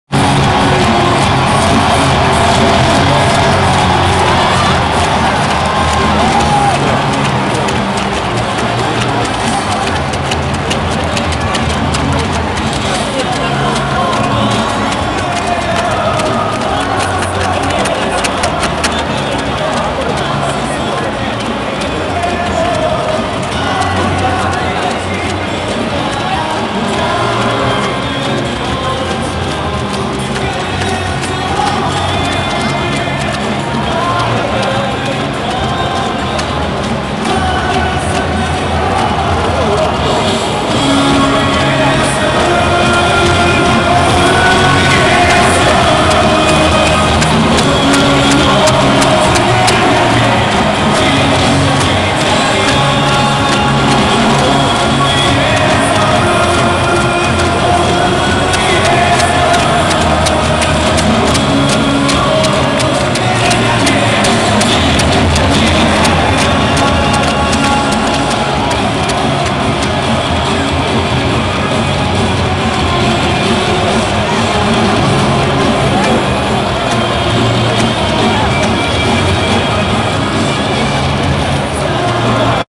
2011 K-League _ Entrance FC Seoul and Suwon BlueWings (서울 vs 수원)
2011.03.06 Sunday - FC Seoul vs Suwon Samsung Bluewings

 it's the 1st round of 2011 K-league and For playing the match, players were enterancing.
as you can see, even they shaked their hand, they took a picture separately.

 드디어 2011 K-리그 제 1라운드가 시작될려고 합니다.
선수들이 입장하여 서로 악수를 나눴지만 이후 기념 사진은 각자 따로 찍고 맙니다.